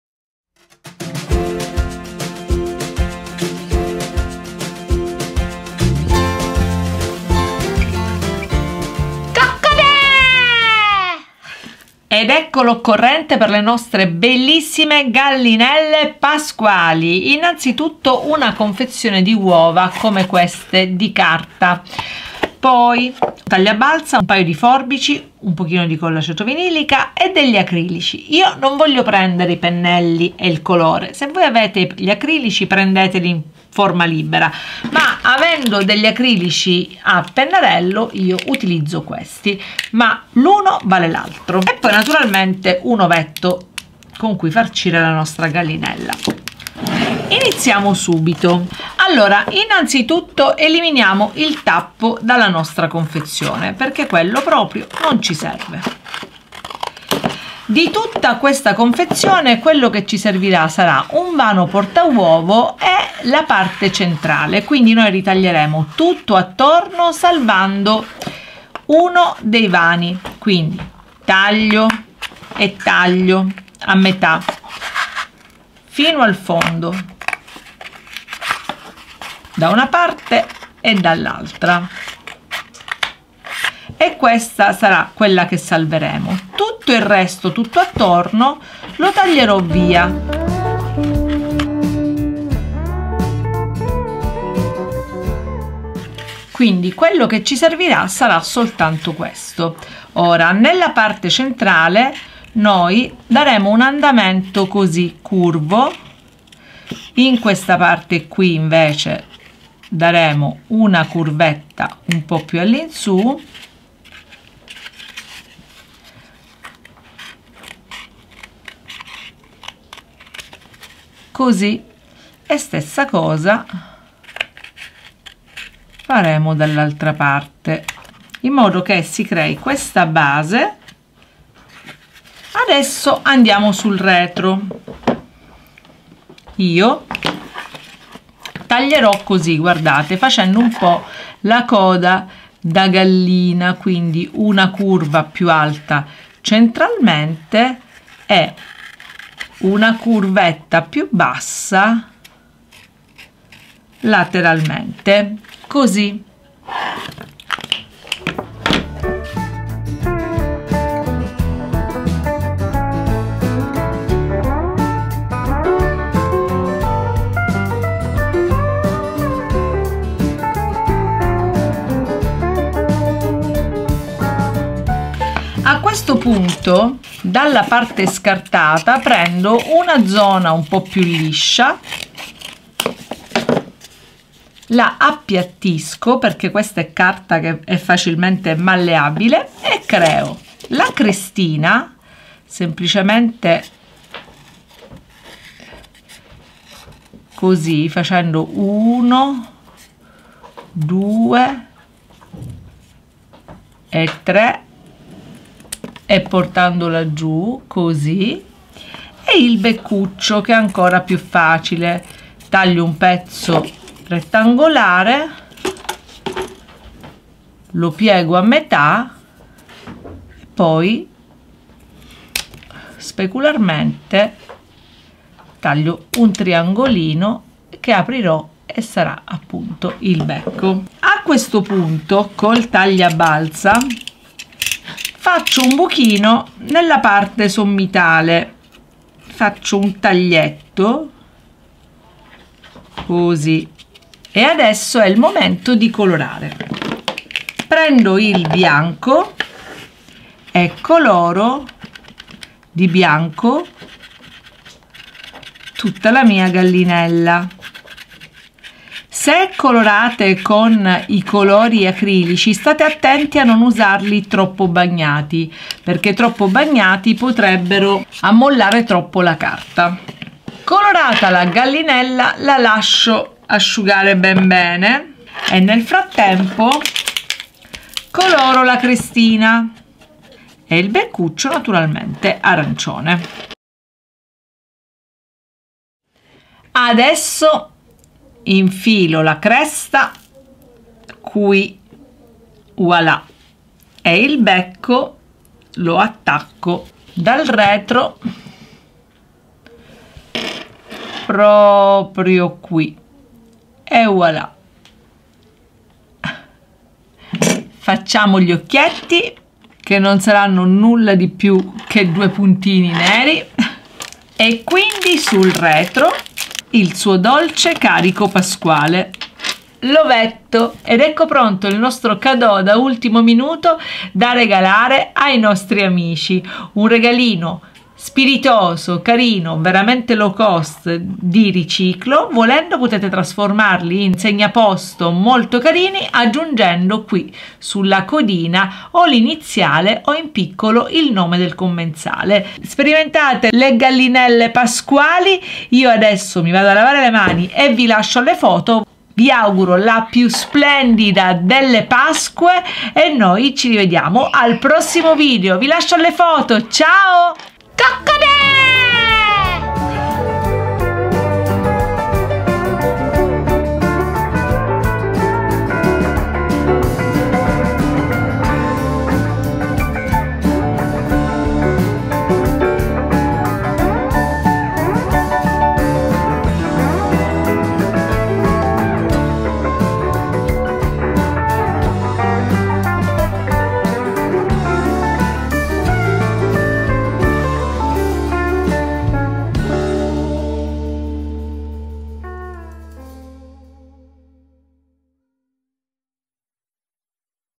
GALLINELLA PORTA UOVO E FELICITÀ! Facilissima per Pasqua! ( Riciclo creativo/ Pasqua) Arte per Te
Ciao a tutti! Oggi vi mostro come creare delle bellissime gallinelle porta ovetto! Carinissime da regalare all’ultimo minuto o semplicemente da usare come porta uovo per le tavole di Pasqua.potete adornarle con uova di zucchero per renderle dei segnaposto pasquali davvero bellissimi! Facilissima da fare e super low cost!￼
Buona preparazione alla Pasqua a tutti e buona visione￼